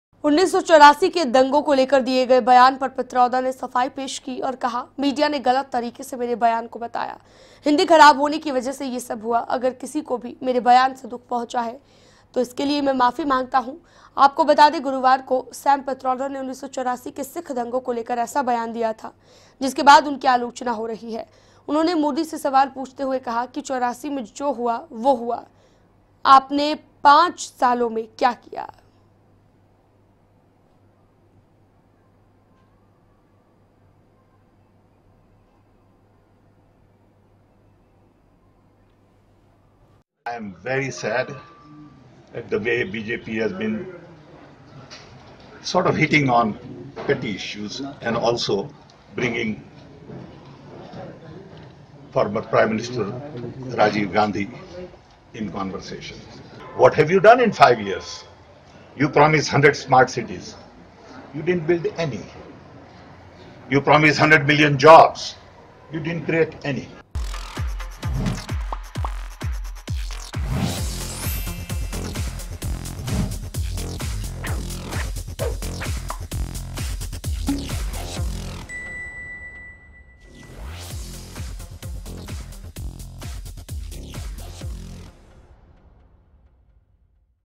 1984 के दंगों को लेकर दिए अपने बयान पर पित्रोदा ने की सफाई पेश || Khabrain Abhi Tak Live
1984 के दंगों को लेकर दिए अपने बयान पर पित्रोदा ने सफाई पेश की है. उन्होंने कहा कि मीडिया ने गलत तरीके से मेरा बयान बताया है. हिंदी खराब होने की वजह से ये सब हुआ है.अगर किसी को भी मेरे ब्यान से दुख पहुंचा है... तो मैं इसके लिए माफी मांगता हूं. बता दें, गुरुवार को सैम पित्रोदा ने 1984 के सिख दंगों को लेकर ऐसा बयान दिया था जिसके बाद उनकी आलोचना हो रही है... उन्होंने मोदी से सवाल पूछते हुए कहा था कि, '84 में जो हुआ वो हुआ... आपने पांच सालों में क्या किया...